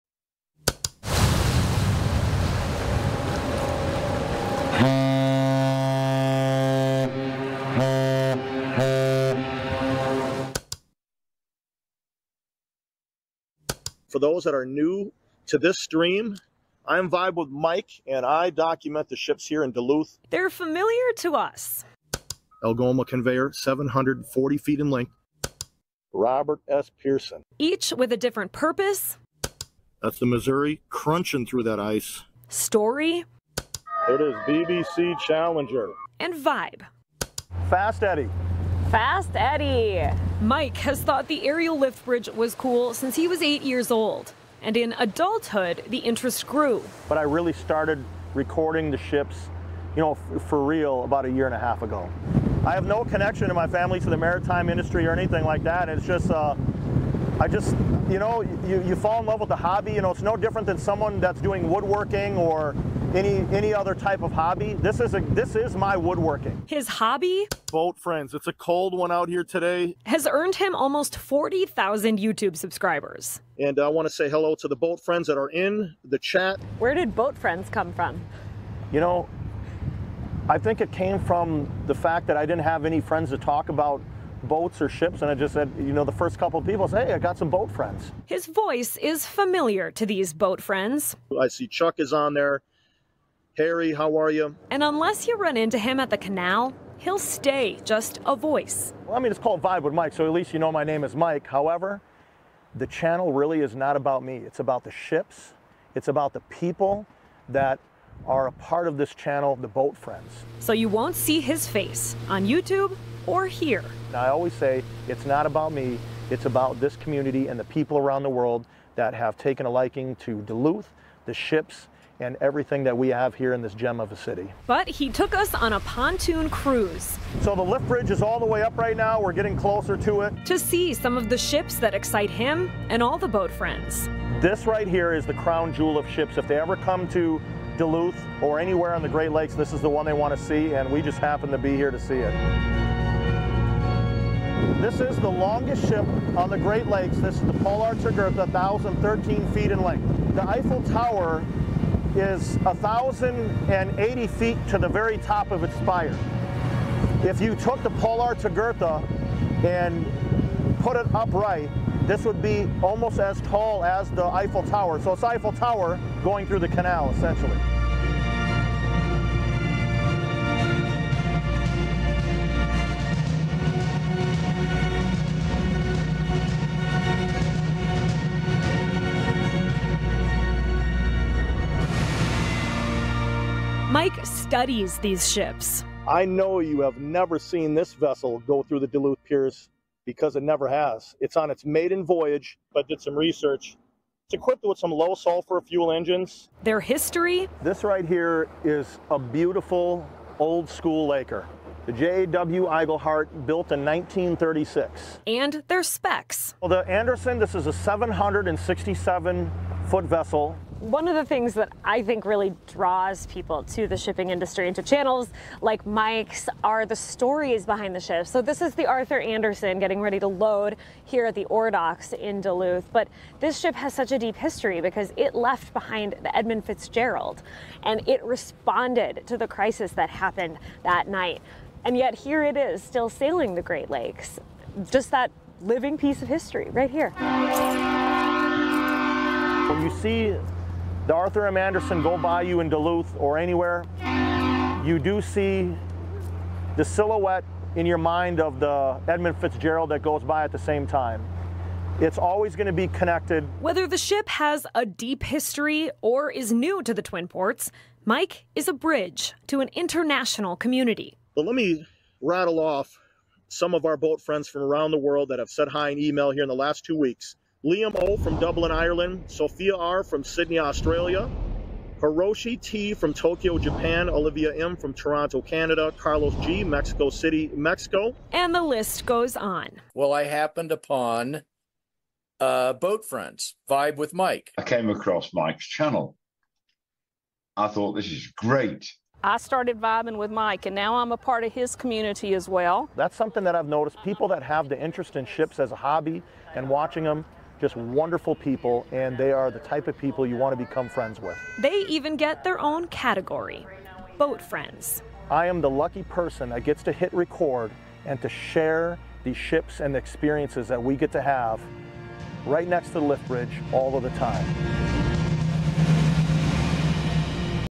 The fascination with shipping is an international experience
For Northlanders, seeing ships come and go from the Port of Duluth-Superior is just a fact of life. But there is an international community online who finds it fascinating and even inspiring.

One way these shipwatchers connect with the Twin Ports is through a YouTube channel called “Vibe with Mike.”

It’s run by Mike, whose passion for Duluth, the Aerial Lift Bridge, and the ships that pass under it started when he was around 8 years old.

“I thought it was so cool, being a kid, you know, being a city guy, all I would see is buses and stuff like that,” Mike said. “As I got older, I’ve just taken a liking to those Great Lakes ships.”

He compares it to any other hobby like woodworking. But his hobby has earned him almost 40,000 subscribers on YouTube. He calls his followers “Boat Friends.”

“YI think it came from the fact that I didn’t have any friends to talk about boats or ships,” he said, “and the first couple people, I said, ‘Hey, I’ve got some boat friends!'”

His voice is very familiar to the Boat Friends. But unless you run into him at the canal, he’ll remain just a voice. He doesn’t ever show his face.

“It’s called Vibe with Mike, so at least you know my name is Mike,” he said. “However, the channel is really not about me. It’s about the ships, it’s about the people that are a part of this channel, the Boat Friends.”

On a pontoon boat ride around the harbor with him in early October, the Paul R. Tregurtha came through the Duluth Canal.

“This right here is the crown jewel of ships. If (the Boat Friends) ever come to Duluth or anywhere on the Great Lakes, this is the one they want to see,” Mike said.

The “PRT” is the longest ship on the Great Lakes at 1,013 feet.

“The Eiffel Tower is 1,080 feet to the very top of the spire. If you took the Paul R. Tregurtha and put it upright, this would be almost as tall as the Eiffel Tower. So it’s the Eiffel Tower going through the Canal, essentially,” Mike said.

He enjoys researching the ships and bringing an educational element to his YouTube videos. One of his favorites is telling the story of the Arthur M. Anderson’s connection to perhaps the most famous Great Lakes ship, the Edmund Fitzgerald.

It was closest to the Fitz when it sank on Nov. 10, 1975. Its captain tried to help look for lost sailors.

“When you see the Arthur M. Anderson go by you in Duluth or anywhere, you see the silhouette in your mind of the Edmund Fitzgerald that goes by at the same time,” Mike said. “It’s always going to be connected.”

You only have to watch a couple of videos to understand the worldwide audience they reach. Mike will rattle off the names of people viewing from Ireland, Australia, Japan, Canada, Mexico, and all around the United States.

“I am the lucky person that gets to hit record and to share these ships and experiences that we get to have right next to the Lift Bridge all the time,” Mike said.

On Tuesday and Wednesday, The Lift will profile a couple Boat Friends from around the world: Dave Heilman from Ohio, Stephen Knight from Wales, and Lauren Olson from Dawson, Minn.